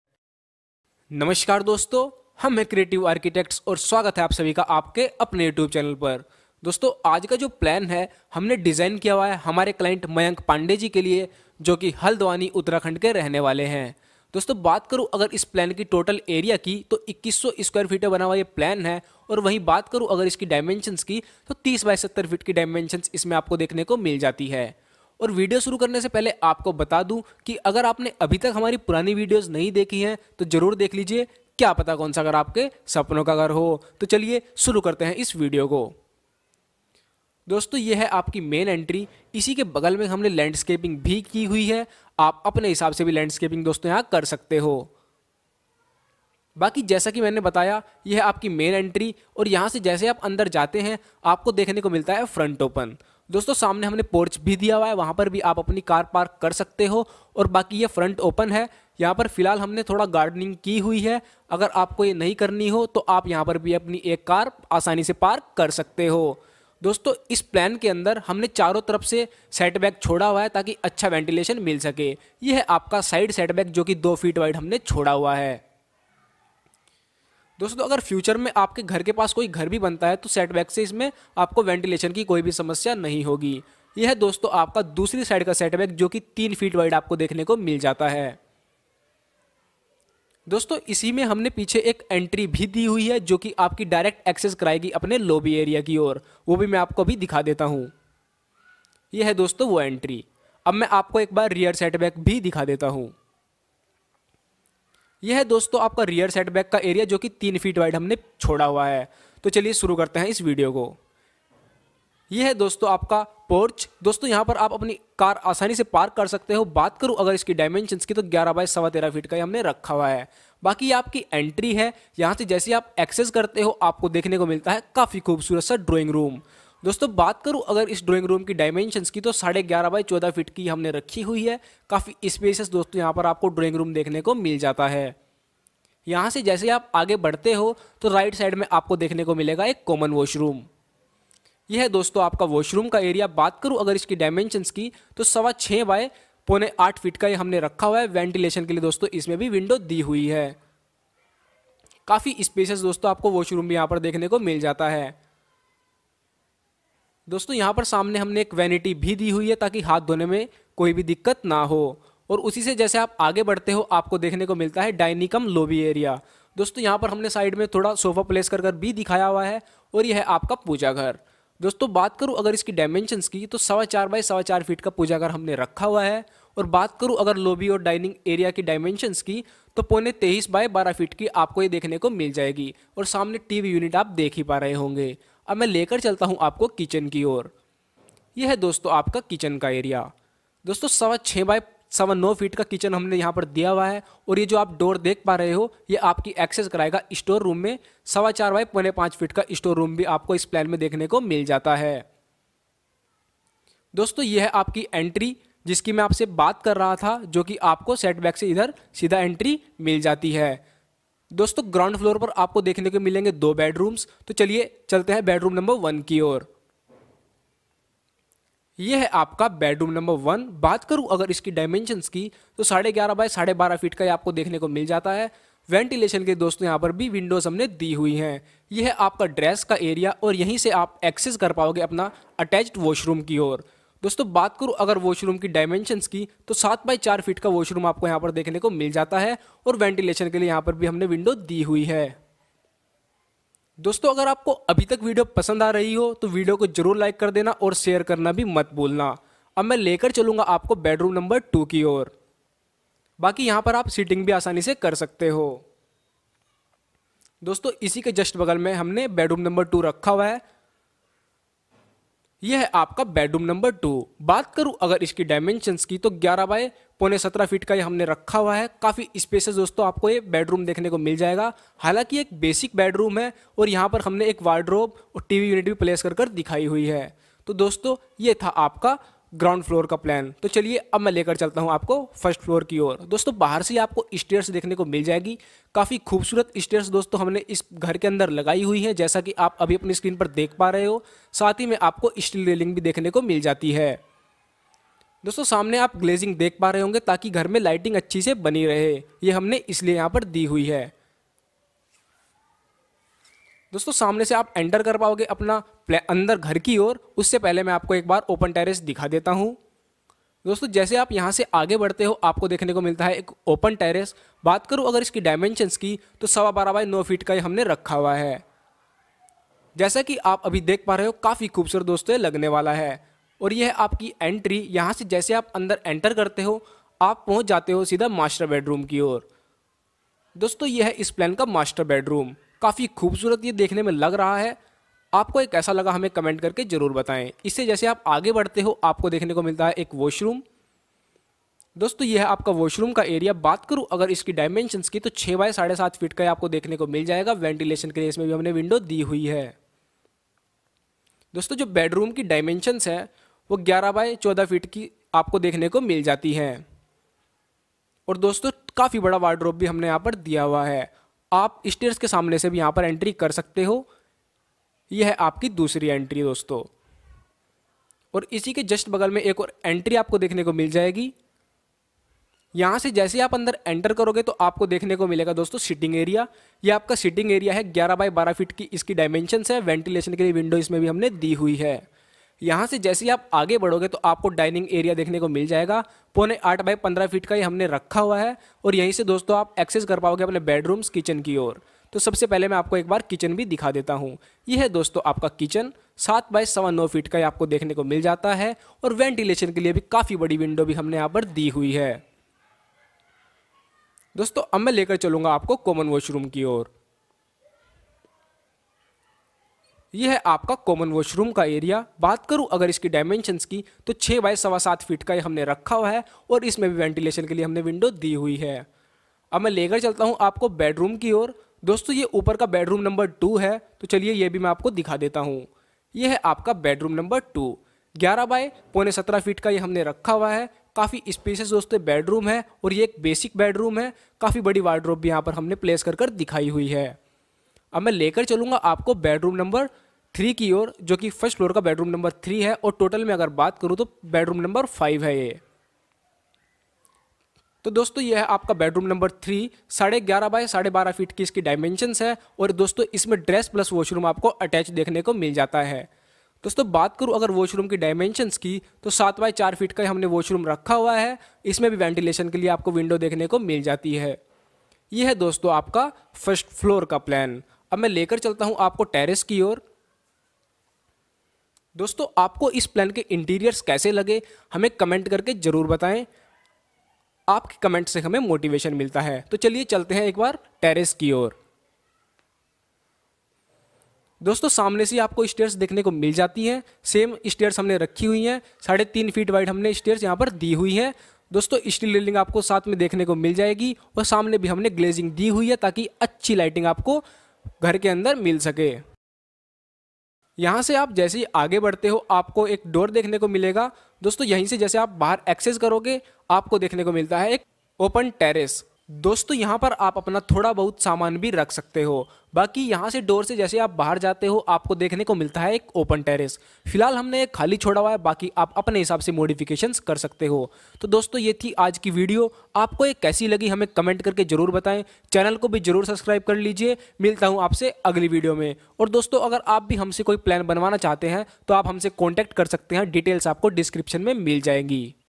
30X70 House Plan 2100 SQFT House Design With Interior in 233 Gaj plot size
Construction Cost - 45 lakh

30X70HousePlan2100SQFTHouseDesignWithInteriorin233Gajplotsize @Creative Architects
In this video we will show you 30X70 Feet  House Design 3D Walk-through, and also exterior view & Interior of the house in 3d. So we hope you guys like this video…